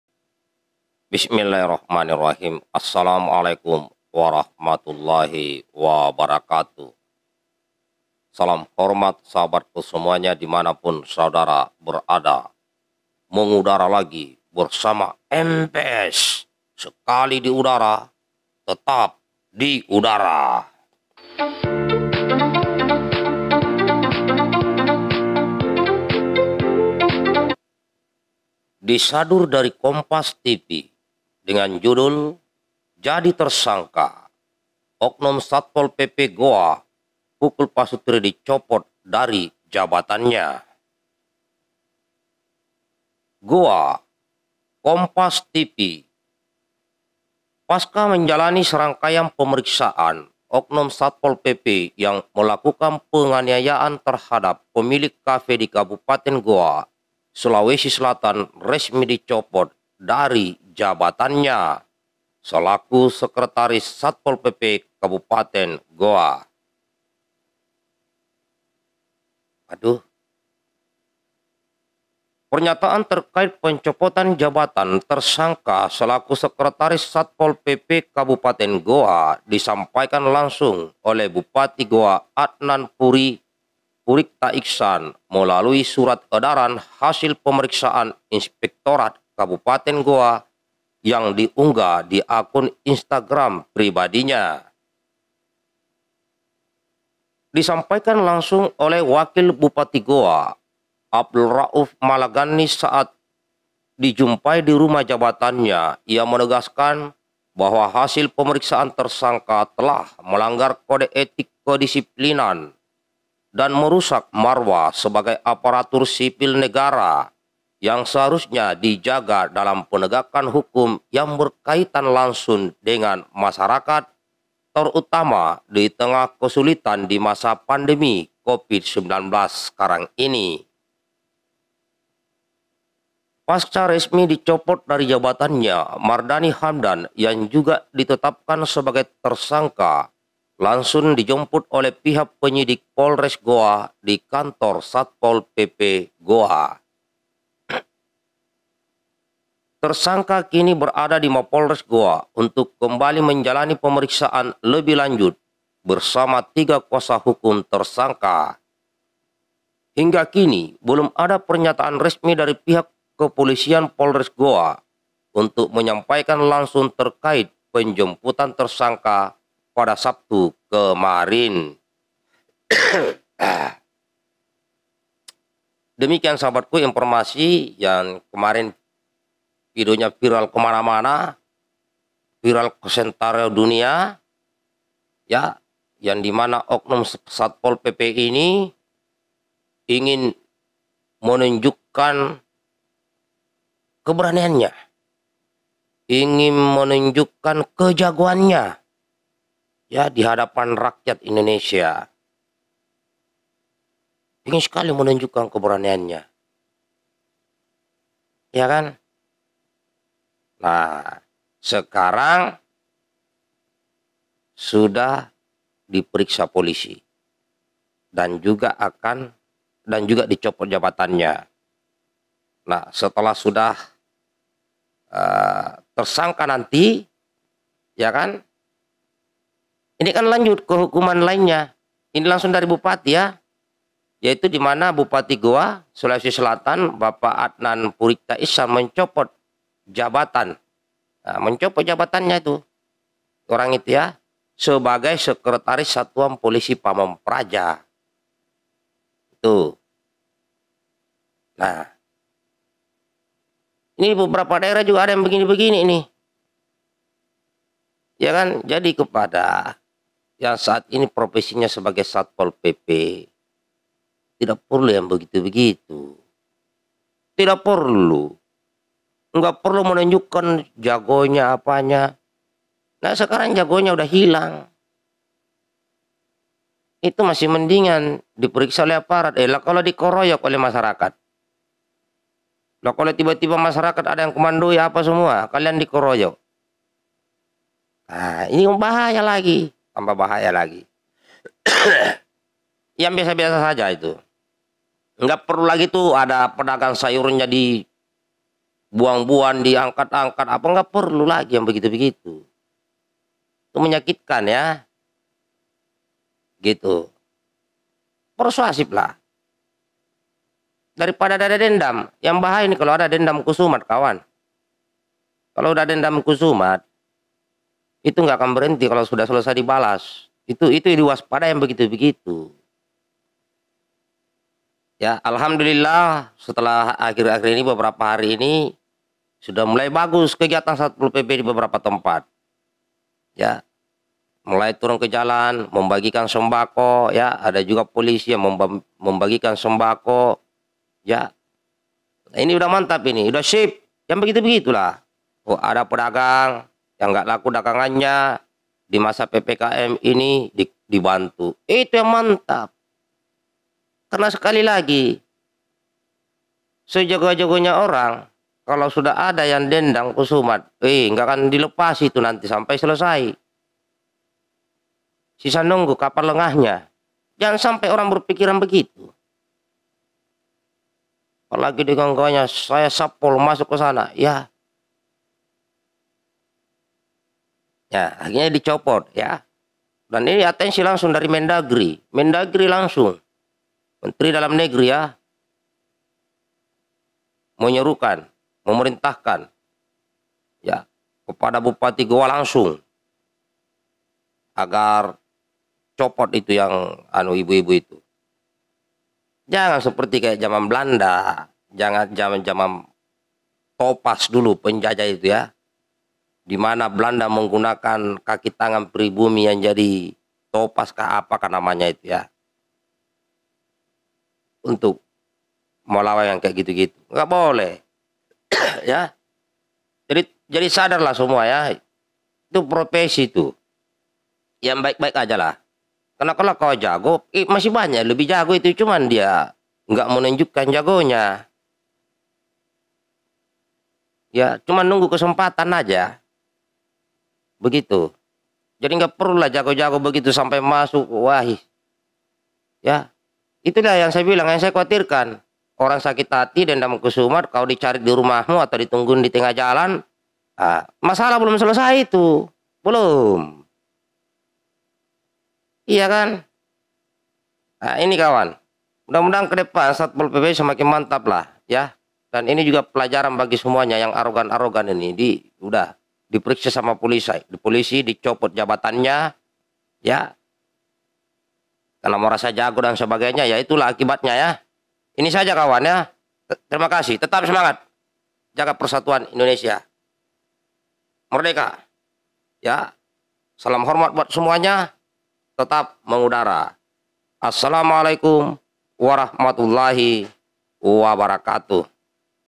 Akhirnya Oknum Satpol PP Gowa yang phukul pasutri warkop di Copot dan jadi tersangka "Saya sappol "
Akhirnya Oknum Satpol PP Gowa yang phukul pasutri warkop di Copot dan jadi tersangka.